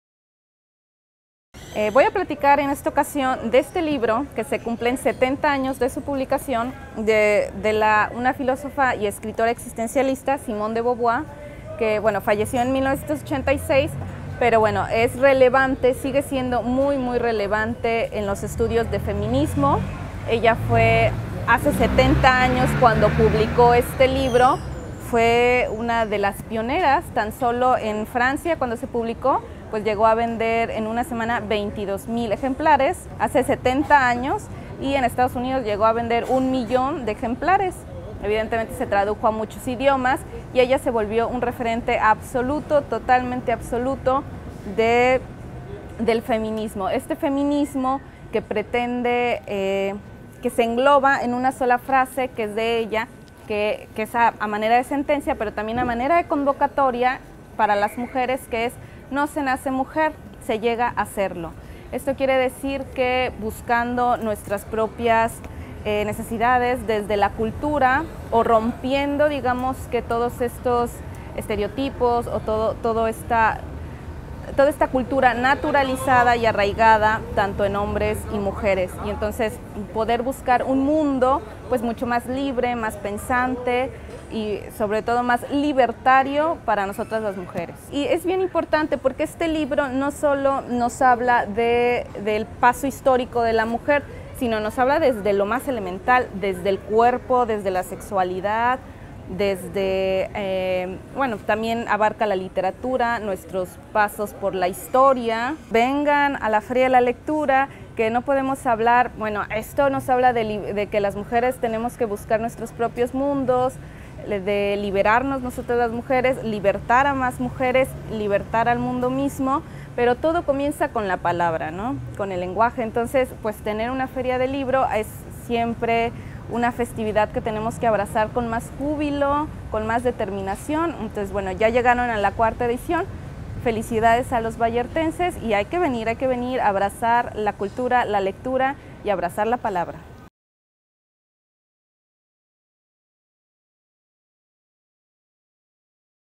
Entrevista a la Lic. Blanca Athié. quien nos hablo de la presentación del libro.
"El segundo sexo, a 70 años de su publicación. El feminismo antes, el feminismo ahora"